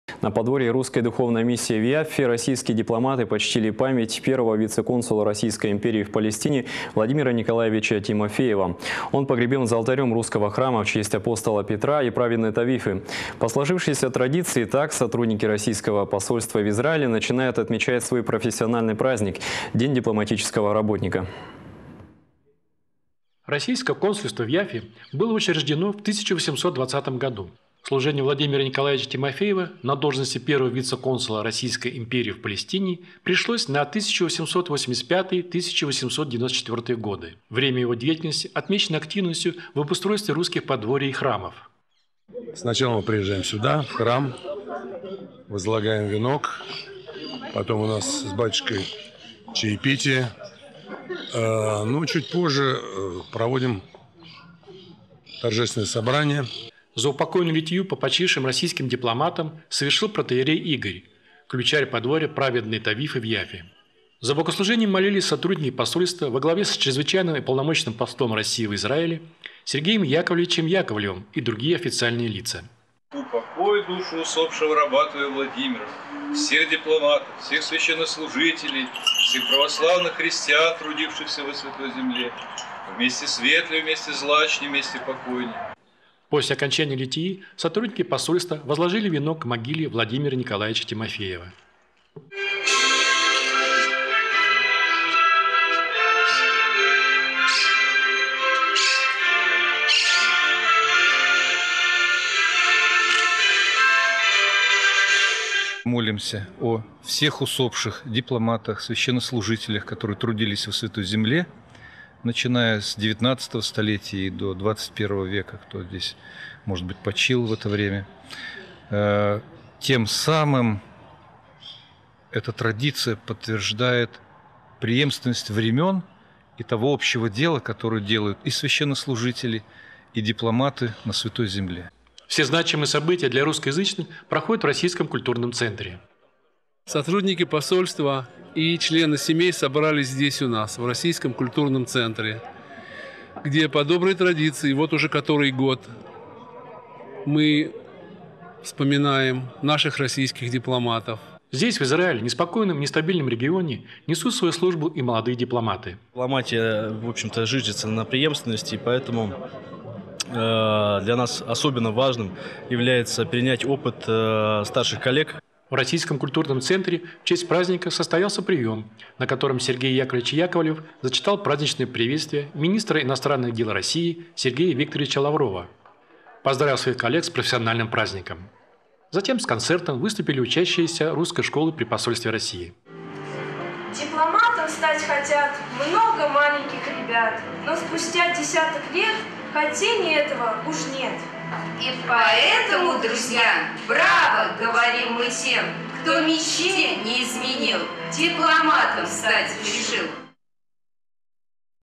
Память В.Н. Тимофеева На Подворье Рус. Миссии в Яффе
На Подворье Русской Духовной Миссии в Яффе российские дипломаты почтили память первого вице-консула Российской империи в Палестине Владимира Николаевича Тимофеева. Он погребен за алтарем русского храма в честь апостола Петра и праведной Тавифы. По сложившейся традиции так сотрудники российского Посольства в Израиле начинают отмечать свой профессиональный праздник - День дипломатического работника.